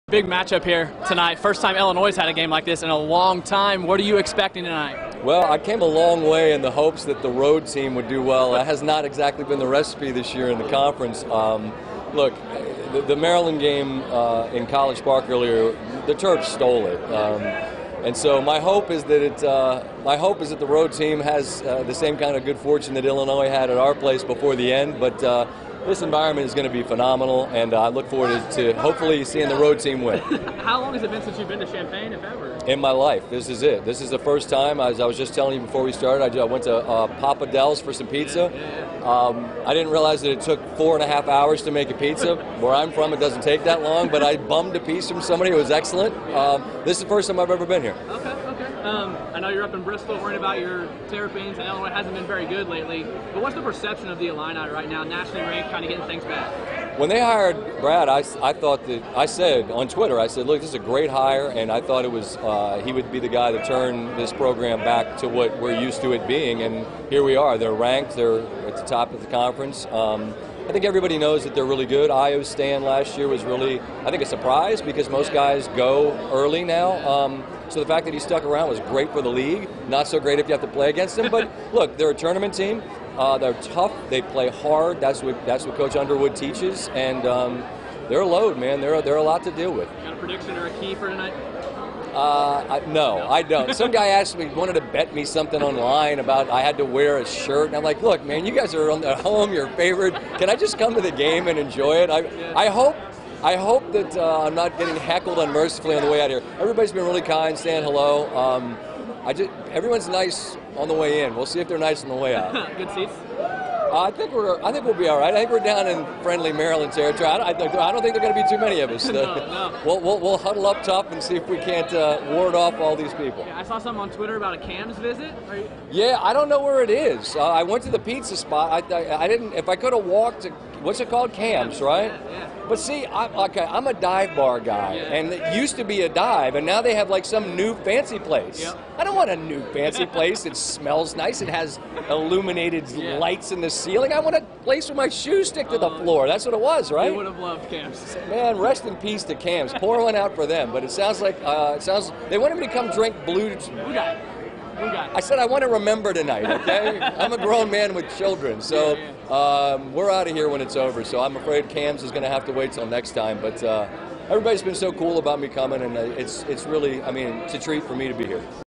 WEB EXTRA: Scott Van Pelt Interview at State Farm Center
CHAMPAIGN (WCIA) -- ESPN anchor Scott Van Pelt talks with WCIA 3's Craig Choate at State Farm Center before No. 20 Illinois hosts No. 9 Maryland.